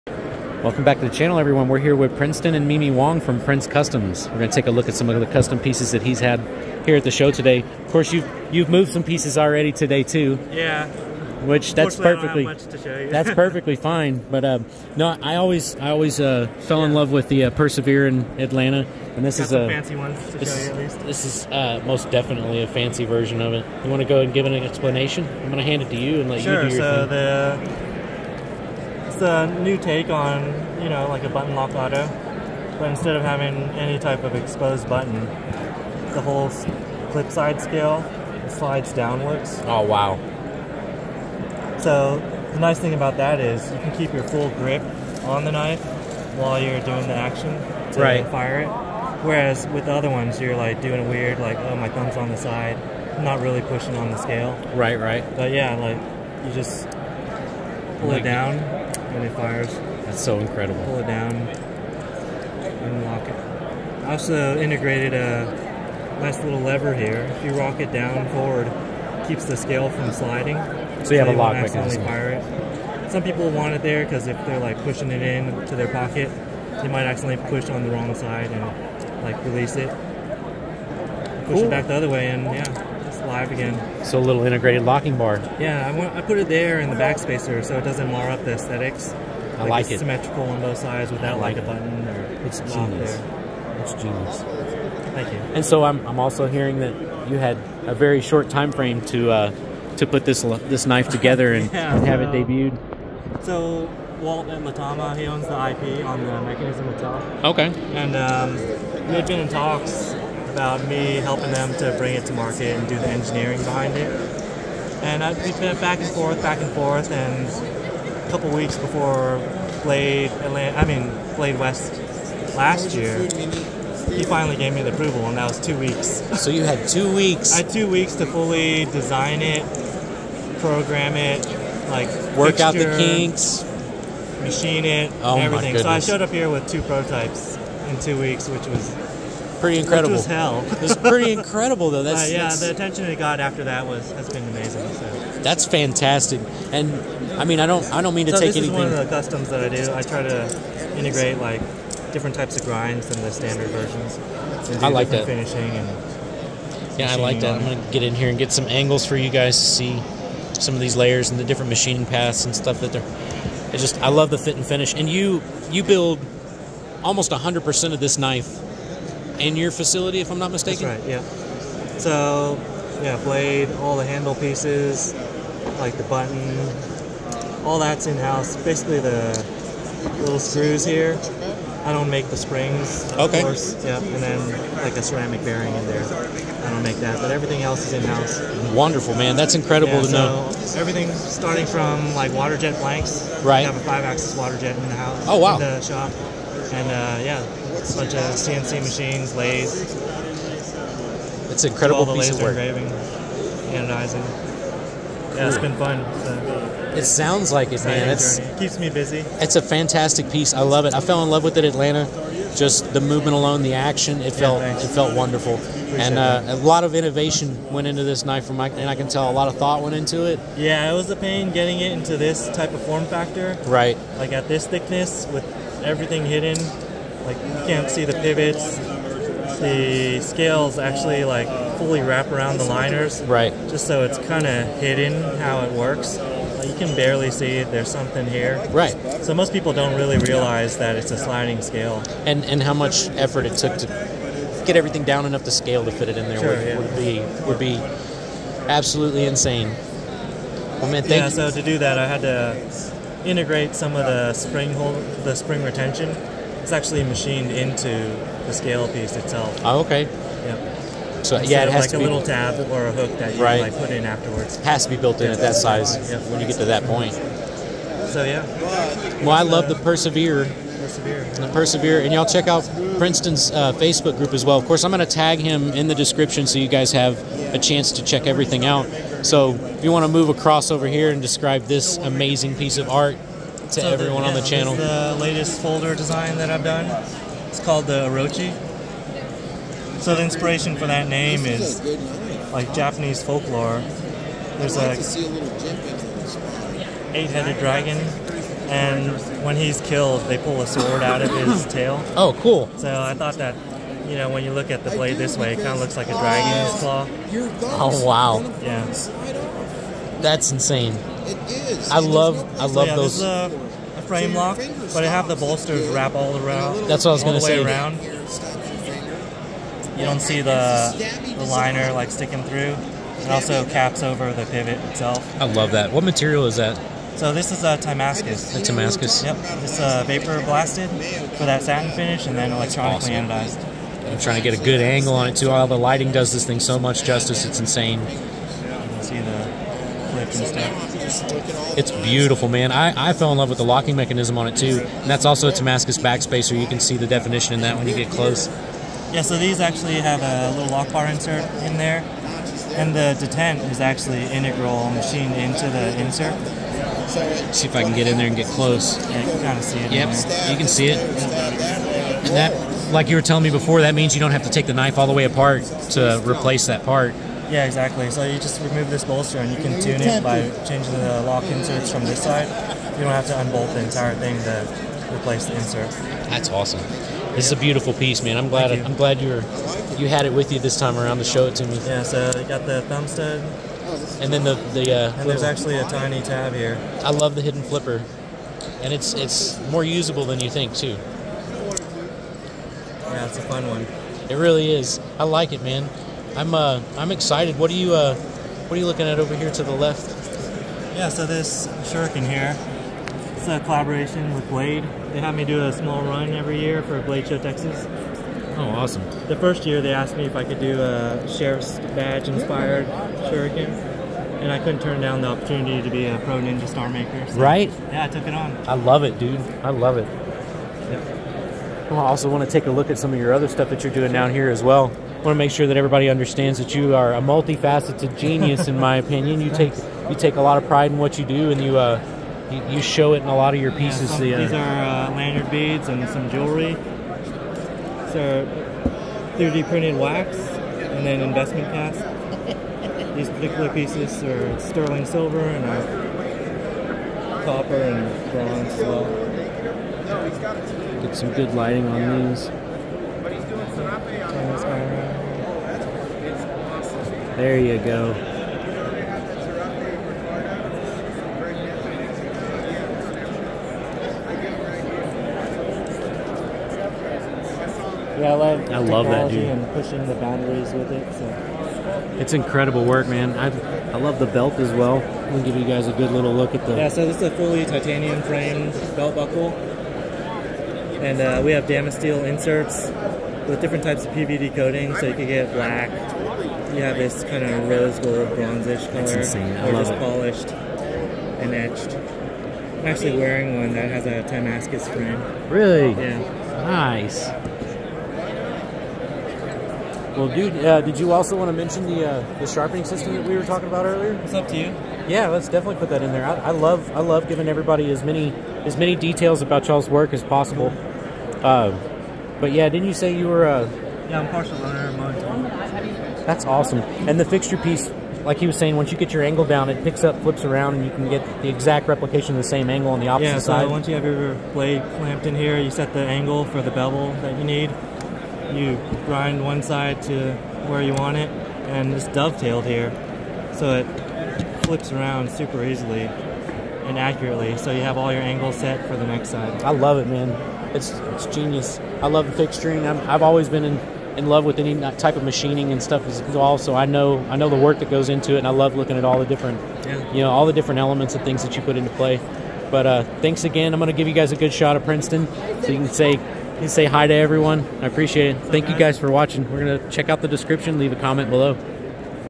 Blade Show West Prince Customs Interview
I was very fortunate to get some time with Princeton Wong of Prince Customs! He explained the Persevere and WAY more!!!